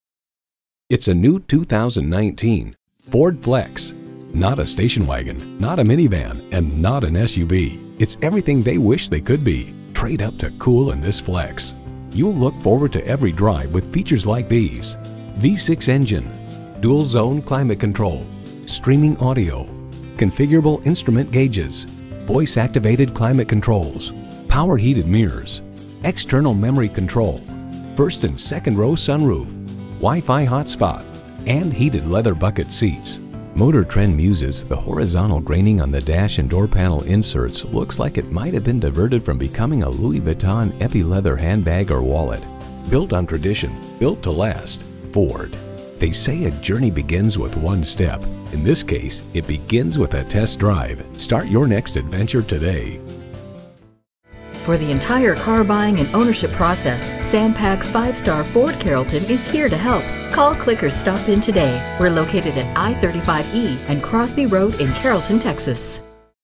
New 2019 Ford Flex Dallas Ft Worth DFW, TX
Year: 2019
Make: Ford
Model: Flex
Trim: Limited
Engine: 6 Cyl. 3.5 L
Transmission: Automatic
Color: White Platinum Metallic Tri-Coat
Interior: Dune
Stock #: KBA18195
VIN: 2FMGK5D84KBA18195
2019 Ford Flex Limited

Address:
1635 Interstate 35